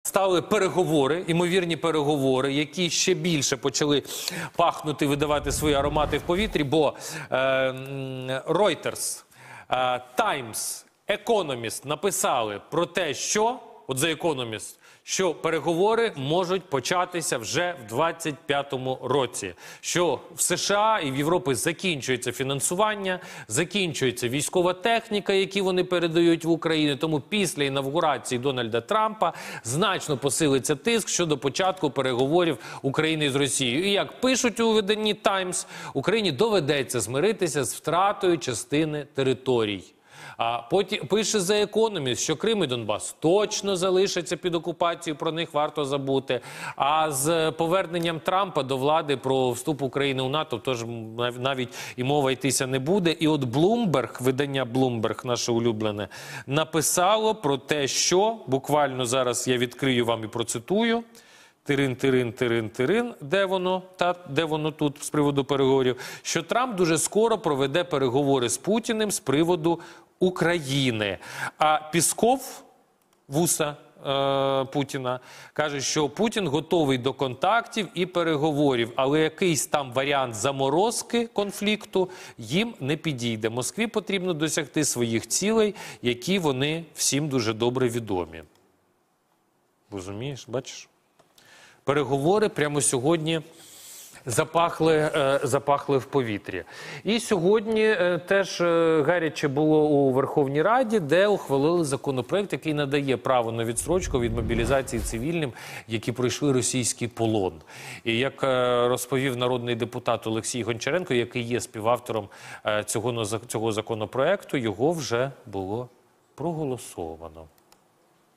Бойові дії в Україні можуть припинитися 2025-го
Україна не здатна власними силами забезпечити армію озброєнням.